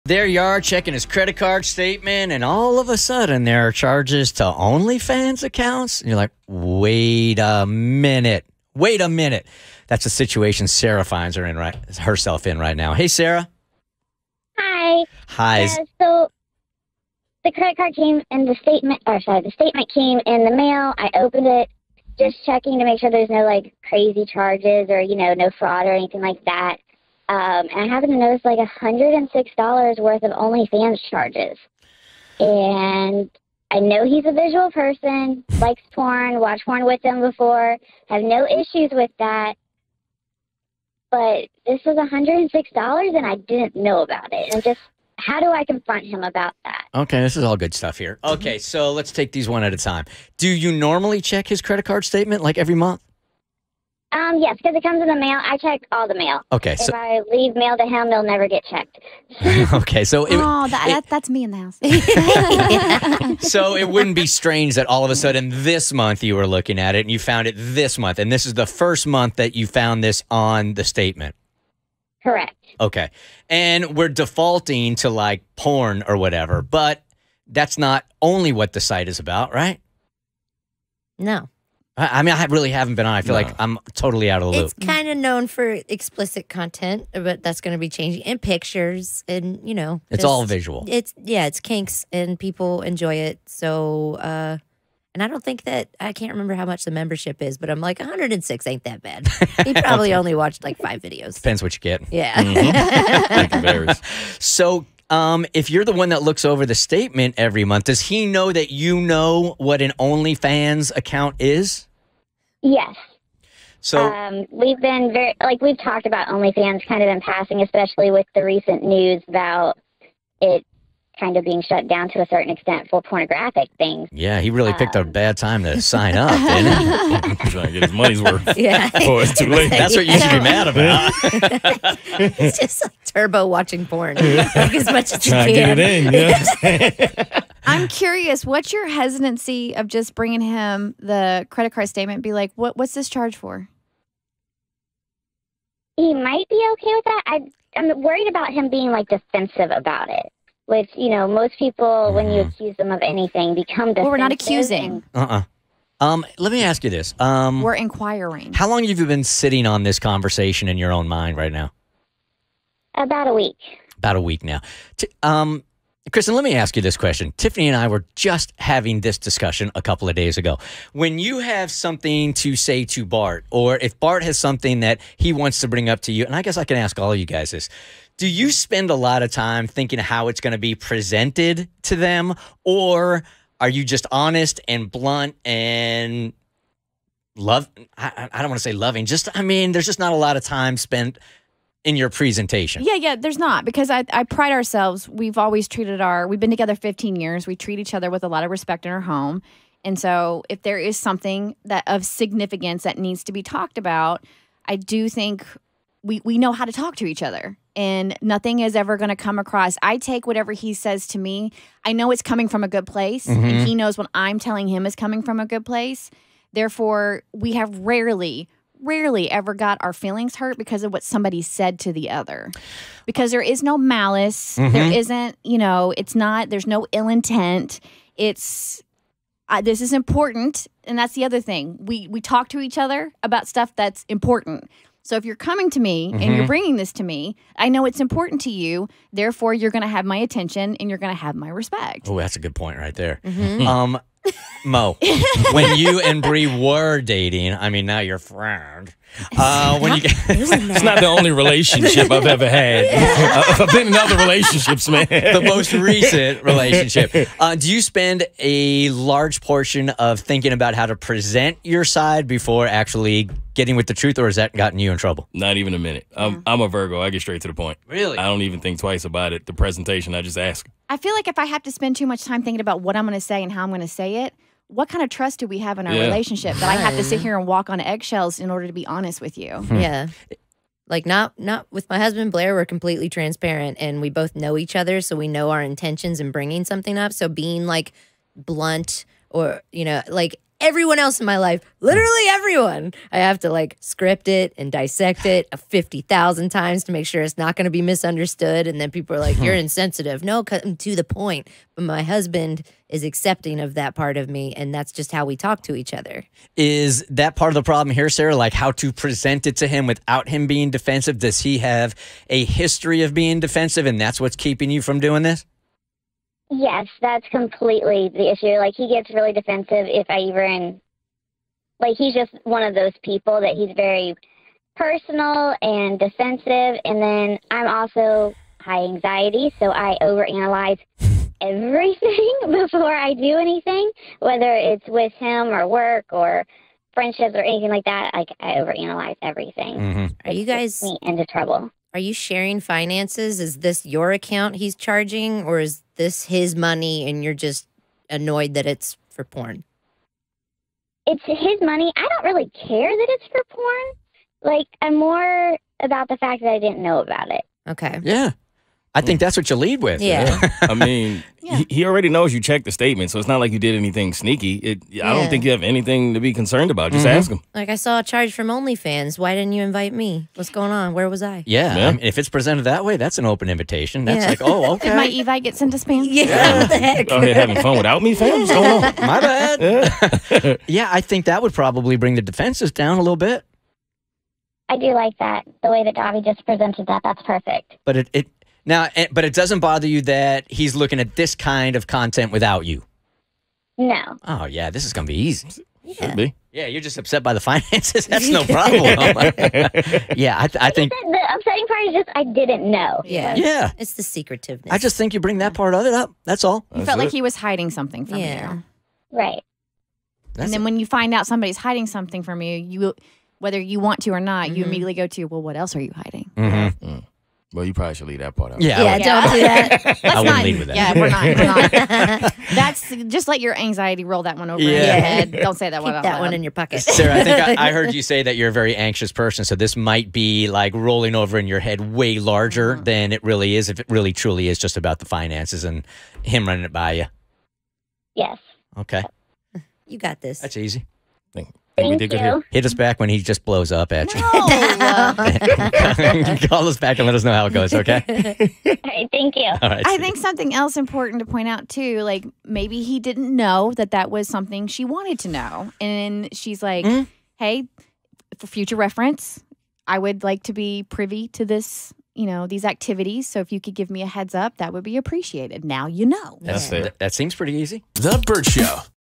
Her Husband Racked Up $600 In Charges On Only Fans!
She found her husband's OnlyFans charges...and it was $600! She had NO CLUE he was on there...should she bring it up to him?!